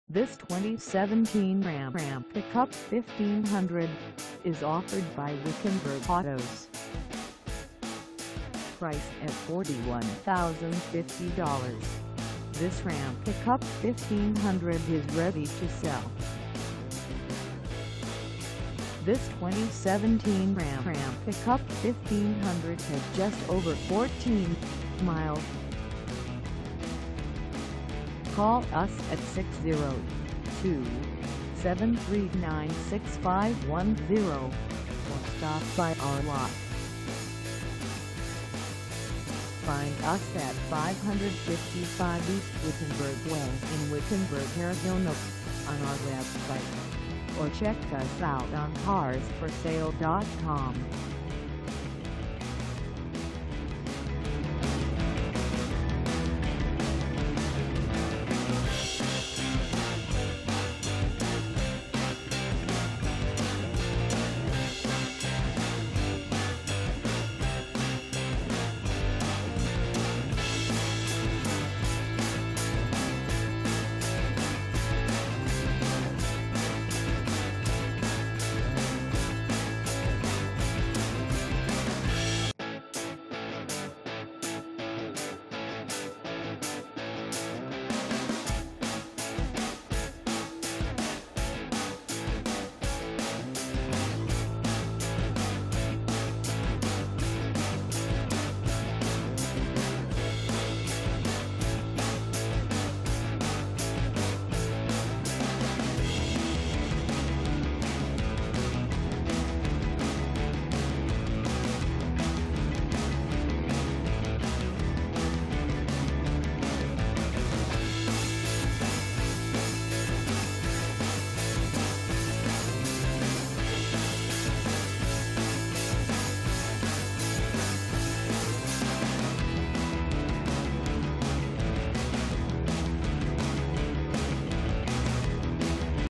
2017 RAM Ram Pickup 1500 Express 4x4 Crew Cab 5'7 Box for sa
This 2017 RAM Ram Pickup 1500 Express 4x4 Crew Cab 5'7 Box is for sale in Wickenburg, AZ 85390 at Wickenburg Autos.

Engine: 5.7L V8
Transmission: Automatic 8-Speed
Color: Brilliant Black Crystal Pearlc
Mileage: 14
Contact Wickenburg Autos 555 E Wickenburg Way Wickenburg, AZ 85390 to test drive this 2017 RAM Ram Pickup 1500 Express 4x4 Crew Cab 5'7 Box today.